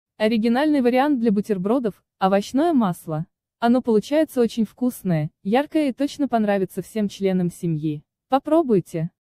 Овощное масло. Классические советские рецепты
Оригинальный вариант для бутербродов - овощное масло. Оно получается очень вкусное, яркое и точно понравится всем членам семьи. Попробуйте!    Овощное масло - прекрасный вариант, как можно сделать ваши бутерброды более разнообразными. Для масла предлагаю взять сладкий болгарский перец, немного моркови, обязательно зелень и специи на ваш вкус. Такое масло отлично подойдет к тостам. Самое главное - выбирайте качественное и вкусное масло.

Ингредиенты:
Масло сливочное — 150 Грамм,
Перец болгарский — 50 Грамм,
Морковь — 50 Грамм,
Укроп — 10 Грамм,
Соль — 1 Щепотка,
Перец — 1 Щепотка,